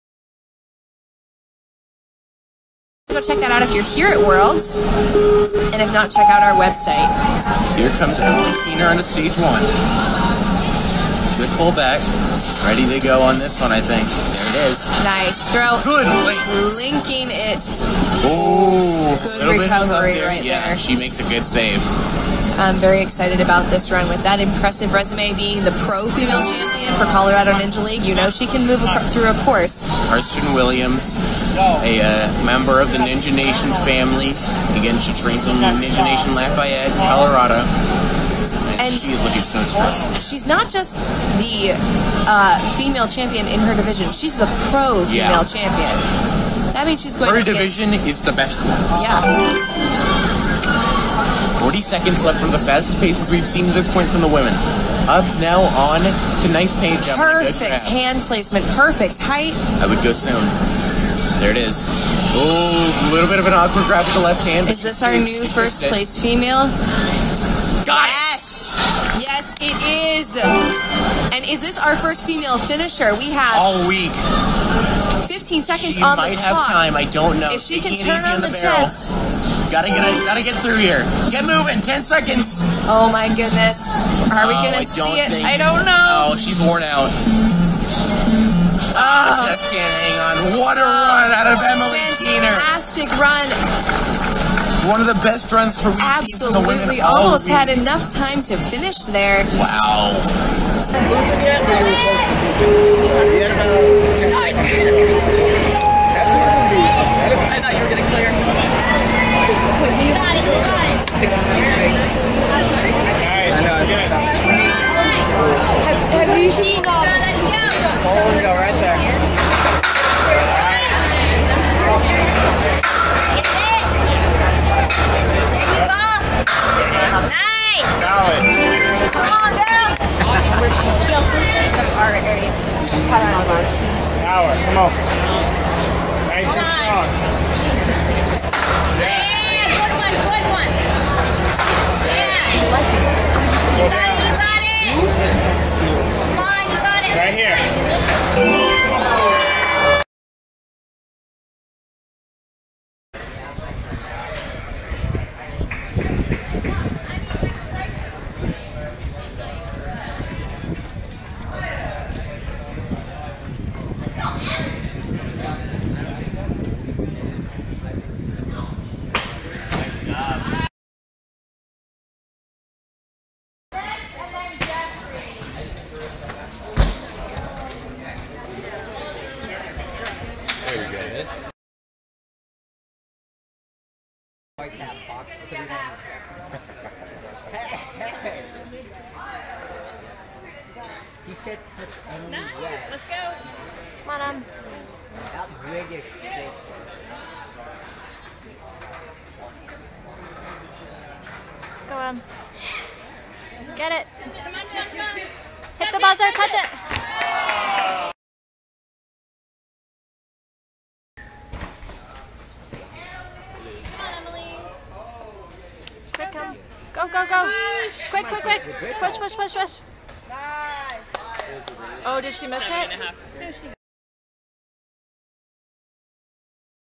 Emily Keener | NNL World Finals 2021 | Courses & Skills
Emily Takes 2nd Place in the Teen Female division at the 2021 National Ninja League Worlds. Watch her course runs as well as all four skills runs (Over the Ledge, Step by Step, Full Swing, and Express Lane).